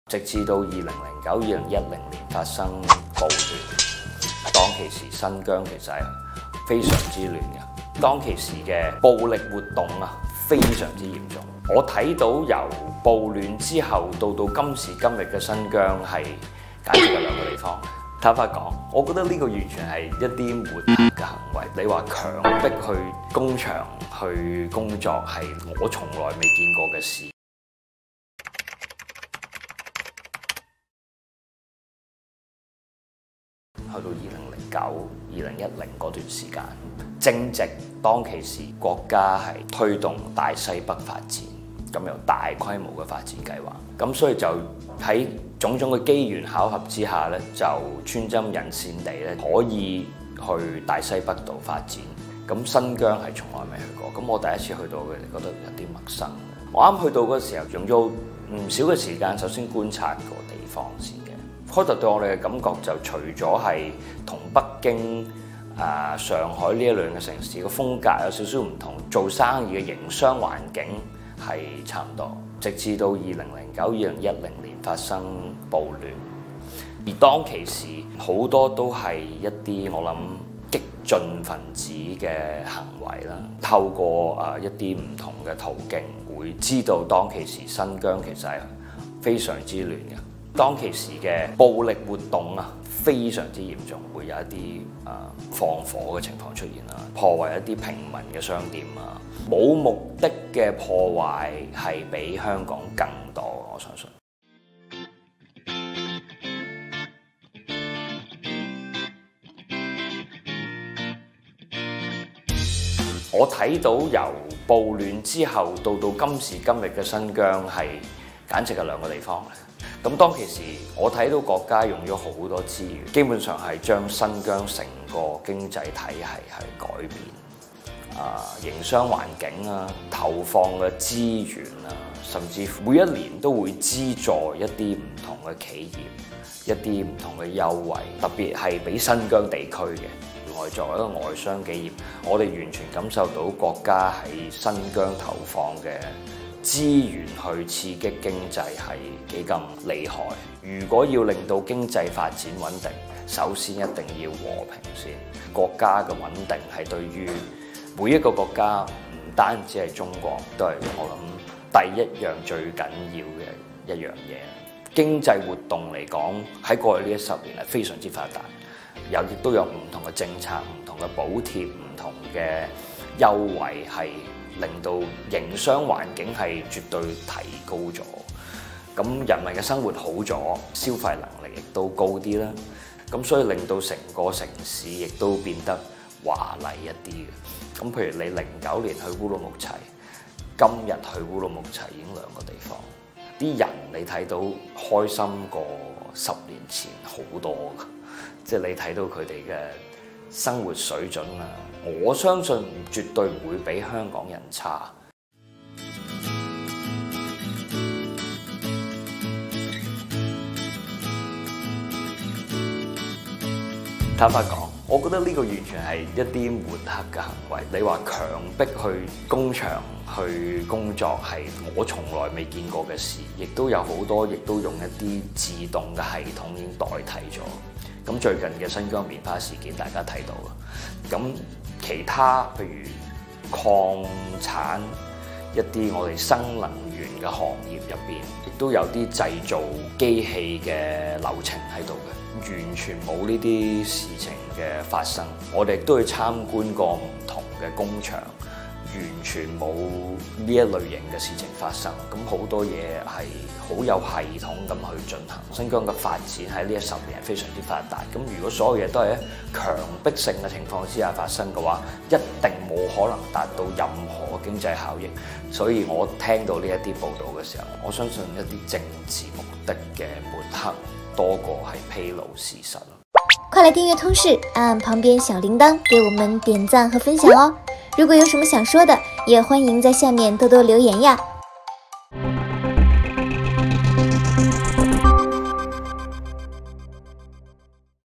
2009年到新疆投資的香港青年：從來沒見過“強迫勞動”這種事
暴亂發生後，國家迅速平亂，並在接下來的十幾年裡，大力發展新疆經濟，解決社會矛盾，改善人民生活。何敬康親眼見證新疆發展，也更懂得和平穩定對社會發展的重要。save hk#新疆